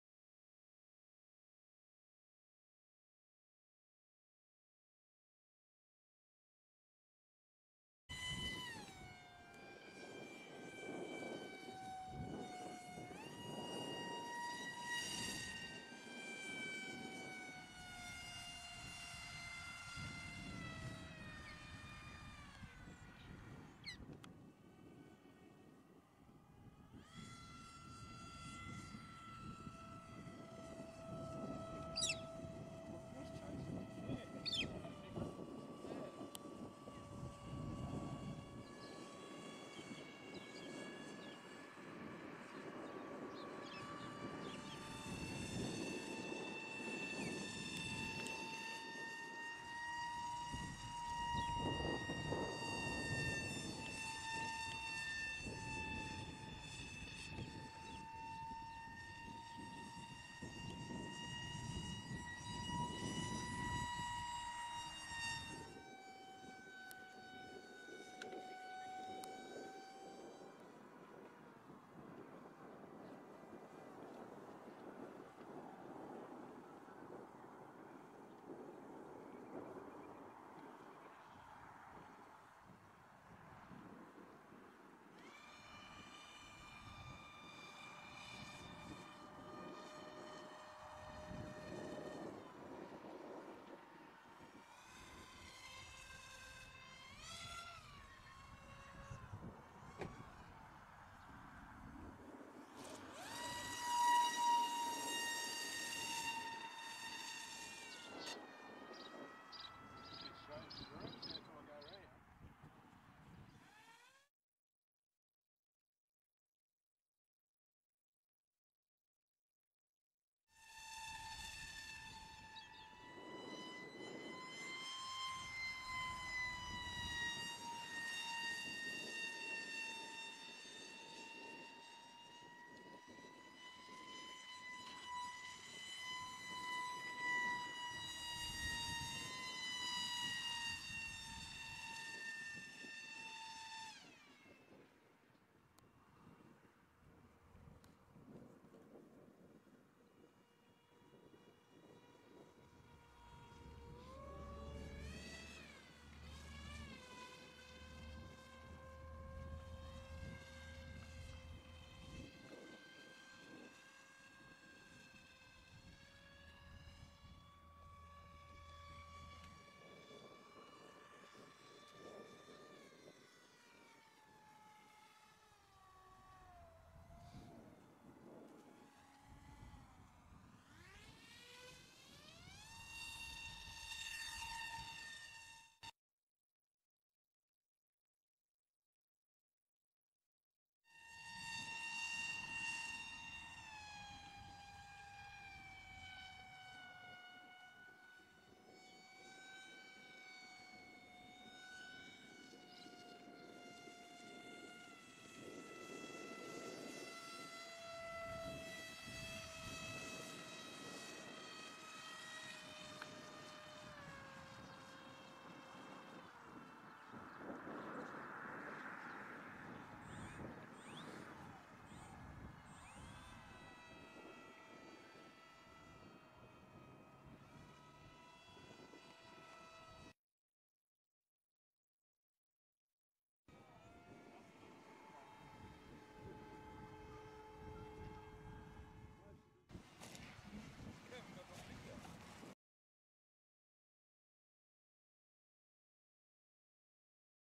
Li'l Eagle biplane (Ground Camera) 22-05-2022
The "Li'l Eagle" modified E-sky Eyas biplane in a bit of aerobatic action.

Ground footage courtesy of Toad Hall Productions (thanks Kermit)